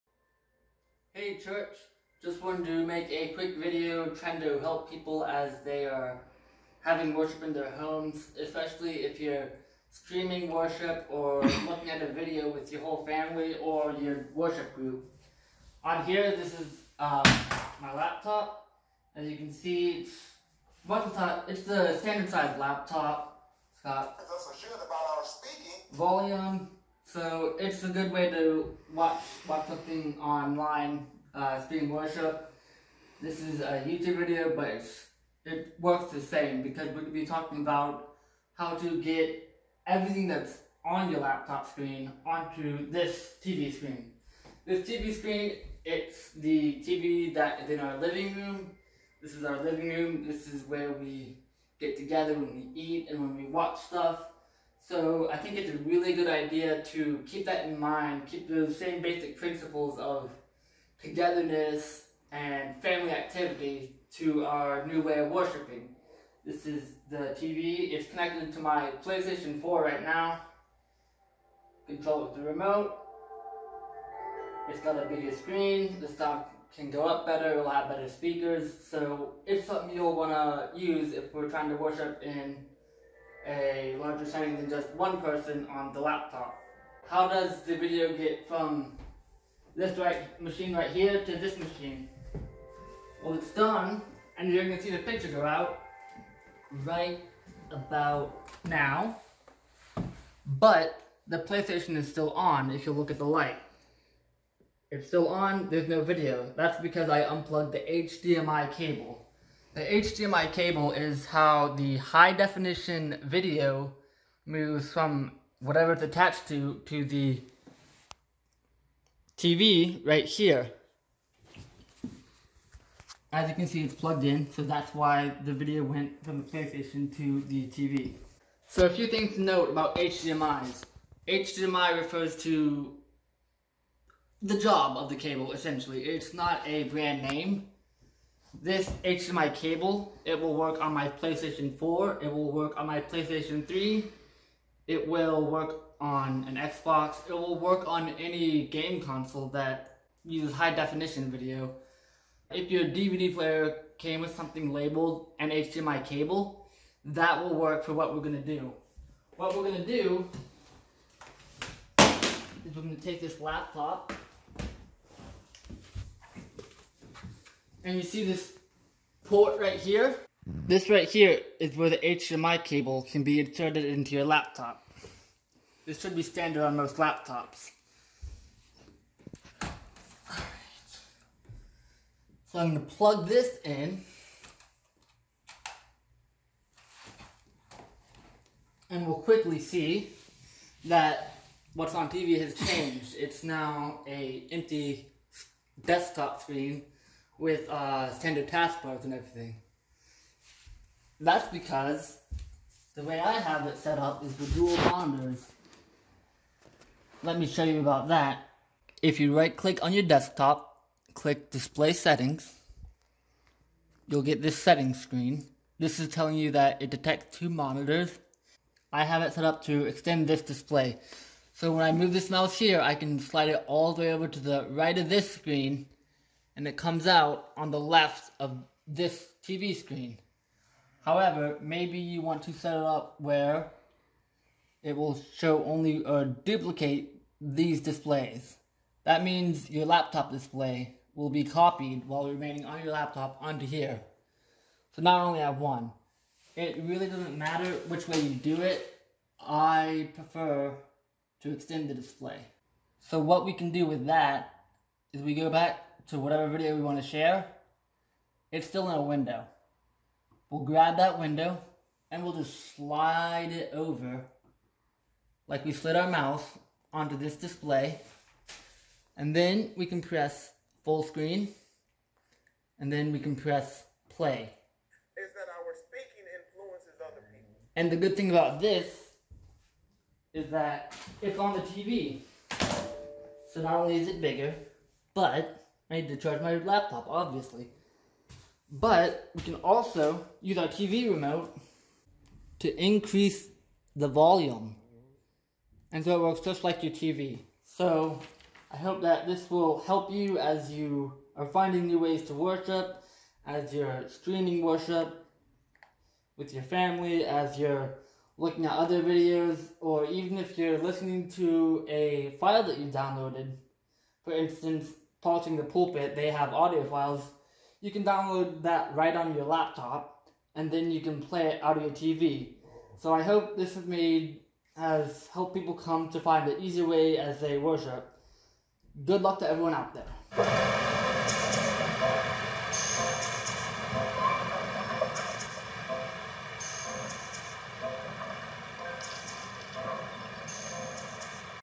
Viewing Videos and Streams on your Living Room TV
We will show you how you can better watch videos with your family or small group. This should make viewing worship streams easier, more accessible, and reinforce that worship is a group activity.